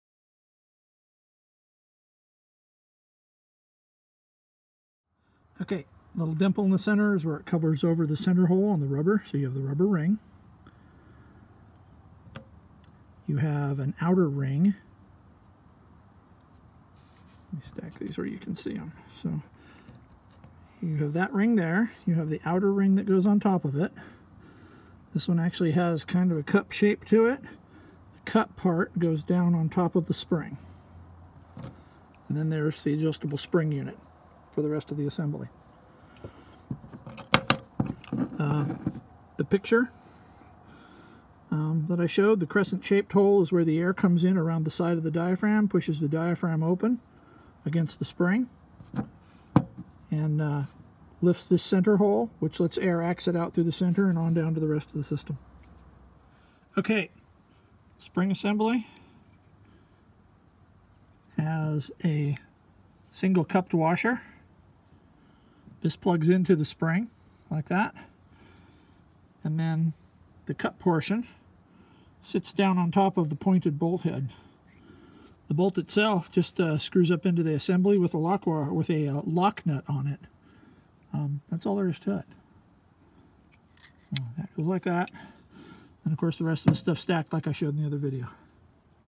LMTV Wet tank air protection valve
85 PSI air protection valve used on the LMTV to feed CTIS and other accessories.